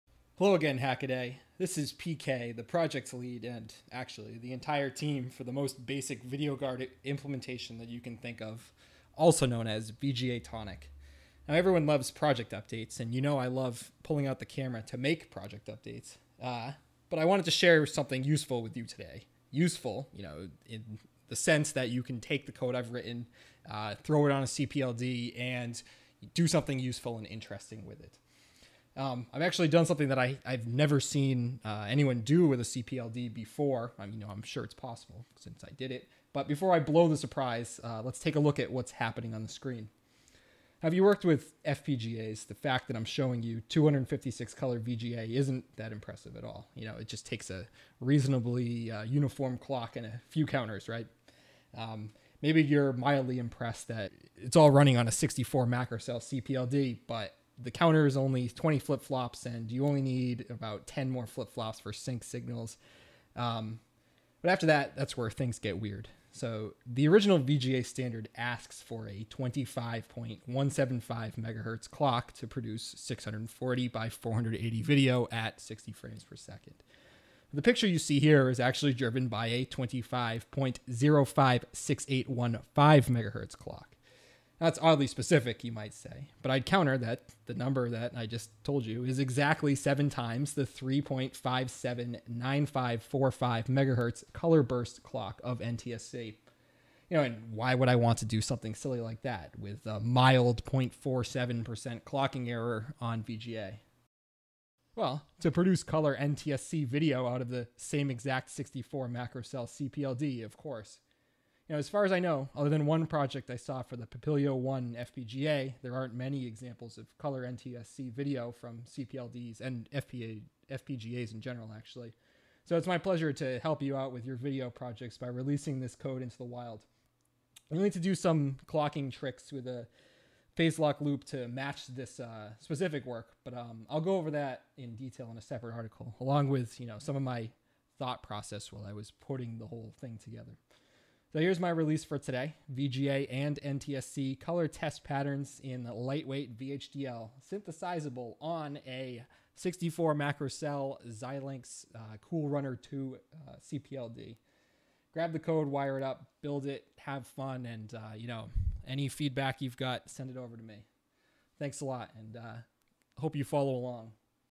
VGATonic - VGA Video on a CPLD
256 Color VGA on a CPLD - then a bonus explained halfway through the video.

Find more details, schematics, and a BOM at the Hackaday Contest project